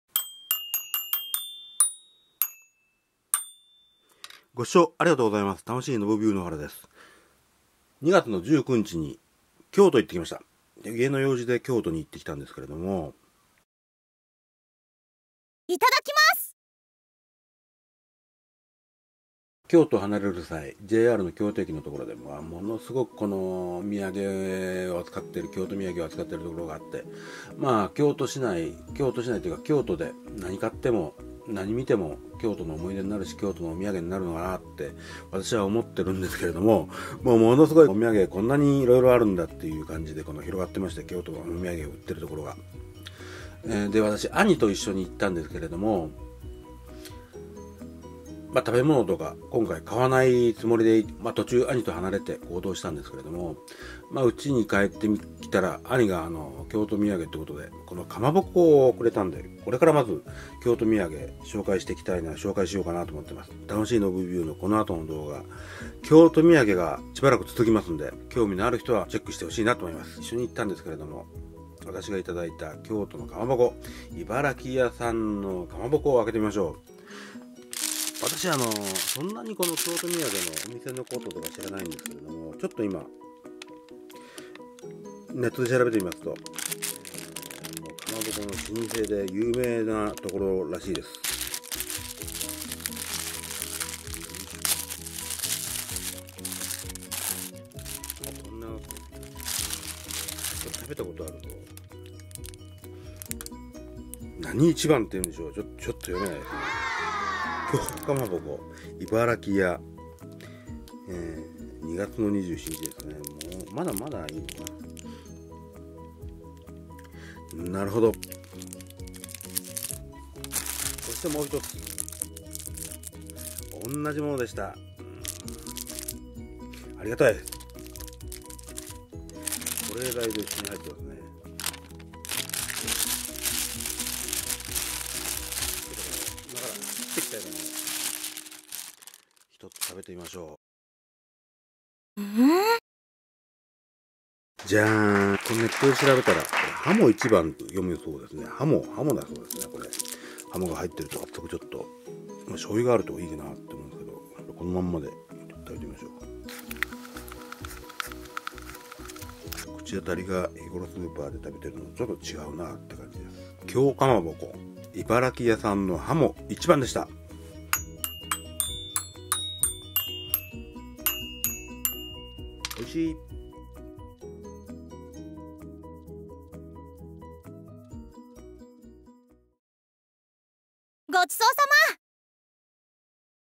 京蒲鉾 茨木屋 鱧一番 : 京都みやげ(私的チョイス）２
2017 2/19 京都に行く機会がありました。
そのときのお土産紹介です。
私的なものなので、言葉は足りませんが、京都みやげの参考になれば幸いです。